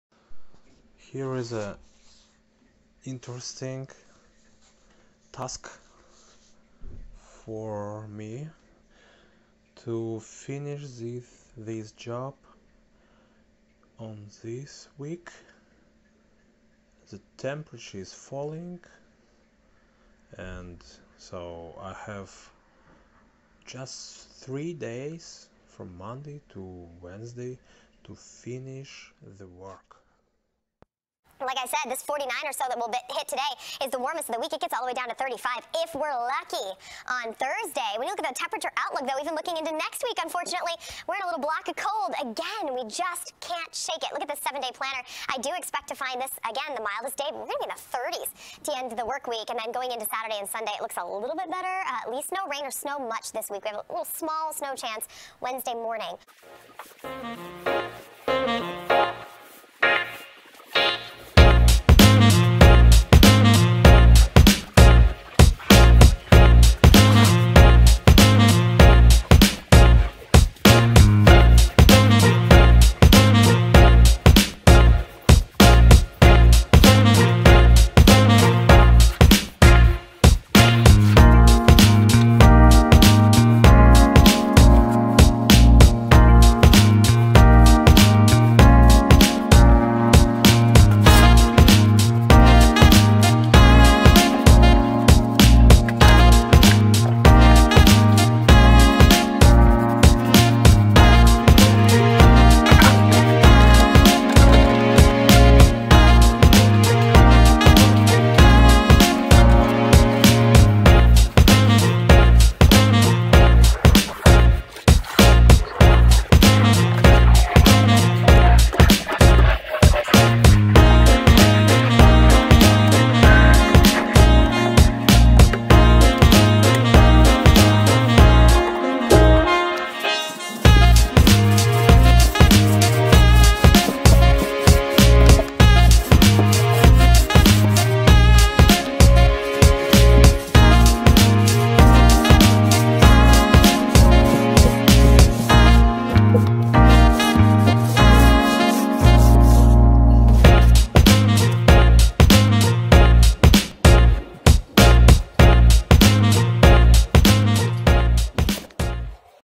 OLD DECK STAINING IN UPTOWN WITH WINTRY WEATHER FORECAST. PART 1
This project is a "blade runner" kind of work. The temperature is falling, and every day can become the winter chilly day. So I have only three days to survive! :-)

If you like this video, please click "like"

Ready to transform the look of your home?

Sign up and like the video!⠀